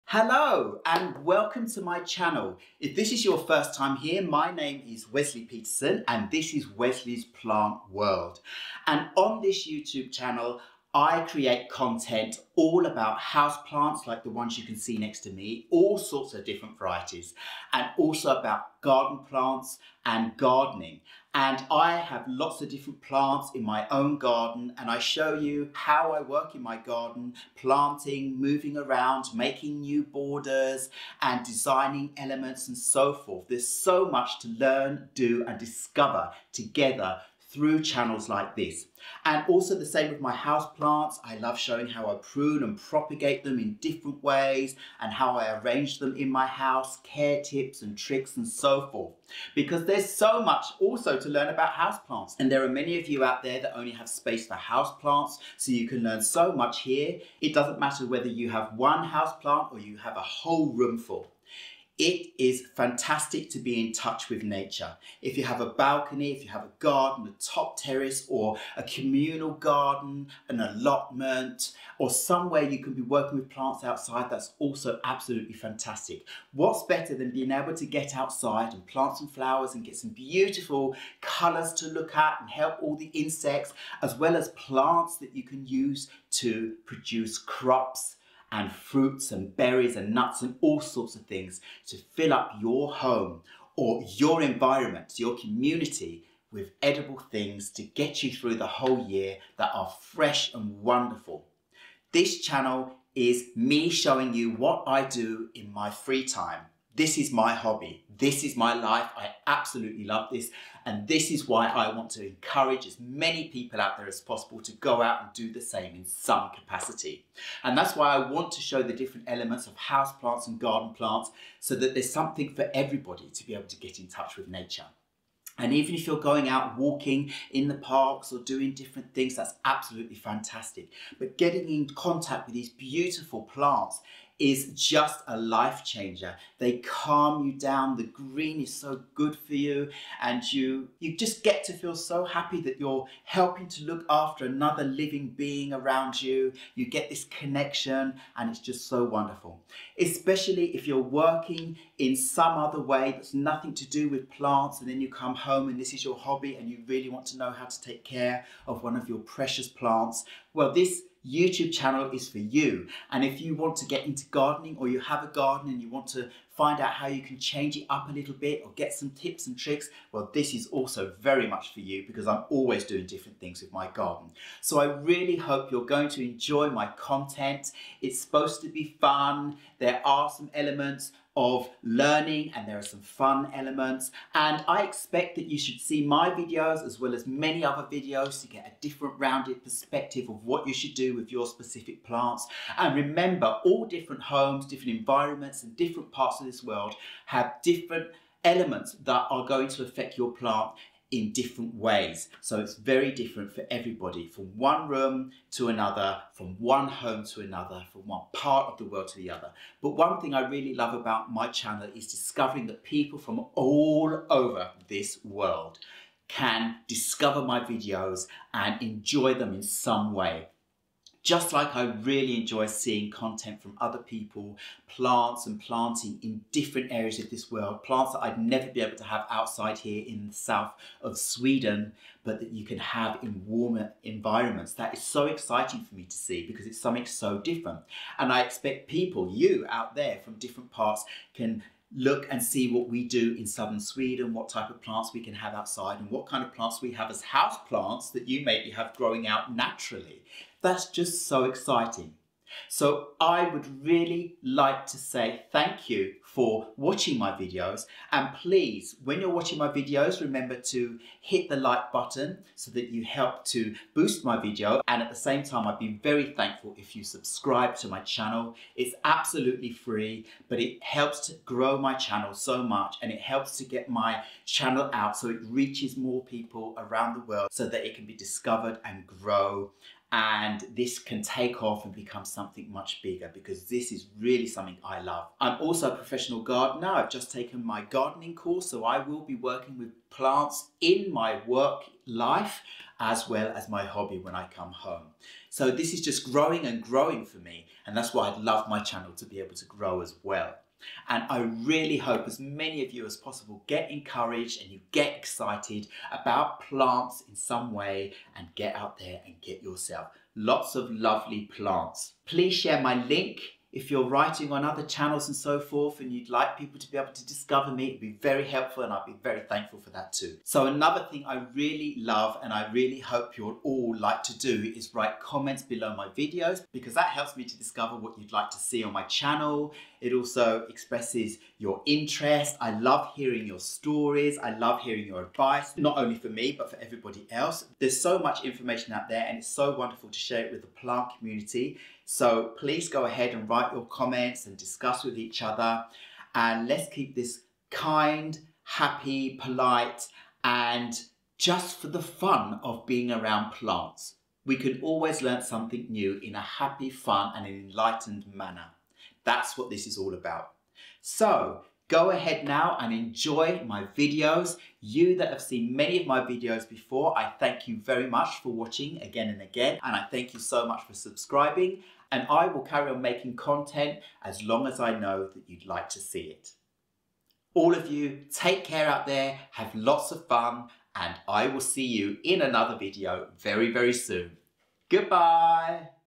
Wesley's Plant World Introduction
Wesley's Plant World Introduction. This channel is for all you houseplant, garden plant and gardening lovers. Enjoy!